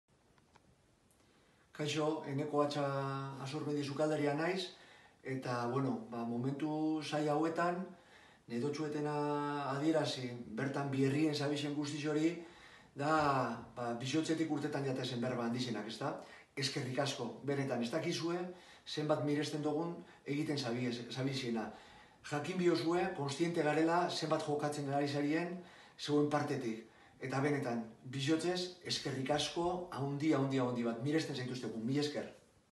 Eneko Atxa. Azurmendi Jatetxeko zukaldaria
Eneko Atxak  musu handi eta animo bat bidali die profesional talde guztiei, eta mirespen guztia.